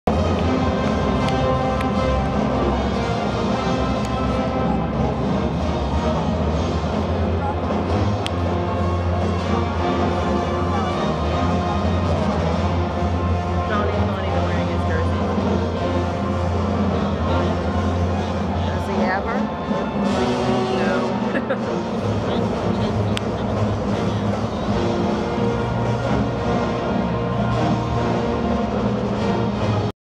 WVU vs. Louisville Cardinals Warm Up 2011
WVU basketball warms up for their win against Louisville